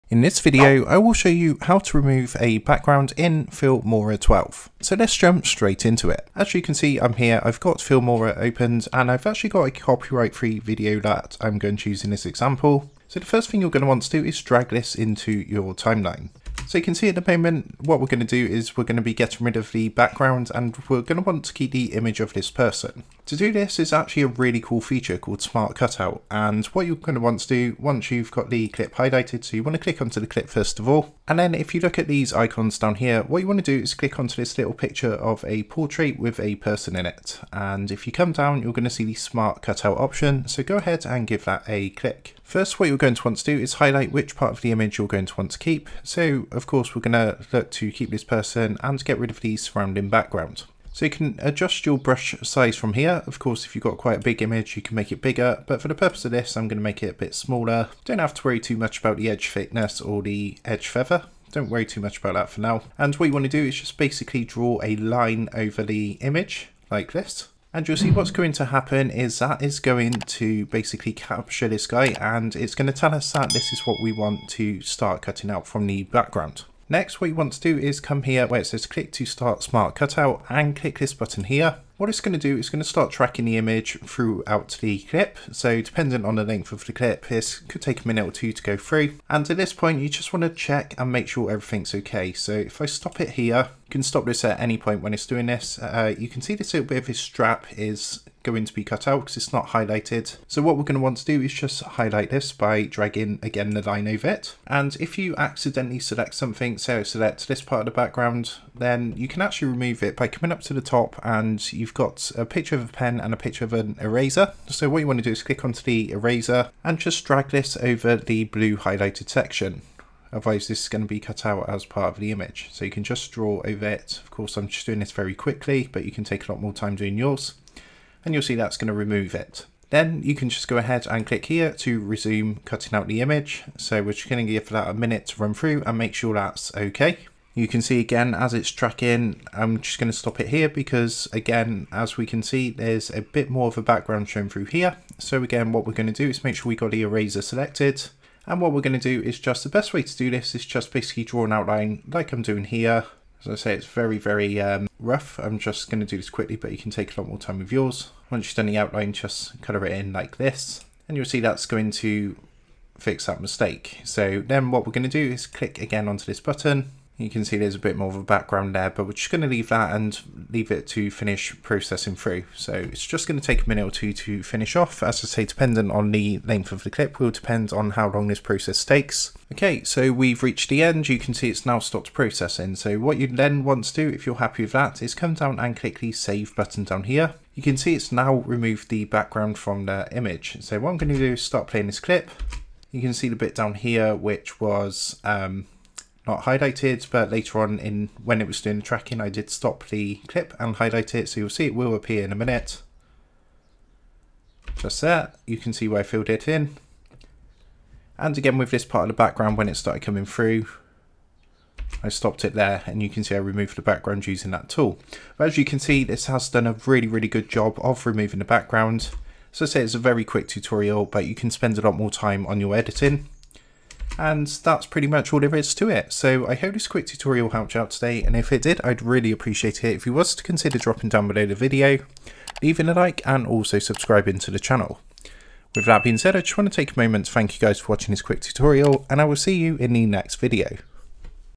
How To Remove Background In Filmora 12
In this tutorial, we'll show you how to remove the background from your videos using Filmora 12. Whether you're looking to create a green screen effect or simply remove an unwanted background, this step-by-step guide will walk you through the process. We'll cover everything from importing your footage to using the chroma key tool to fine-tune your results. With our easy-to-follow instructions, you'll be able to create professional-looking videos with ease. So, if you're ready to take your video editing skills to the next level, join us and learn how to remove background in Filmora 12!

 ✨Recommended VPN: ✨

Thanks!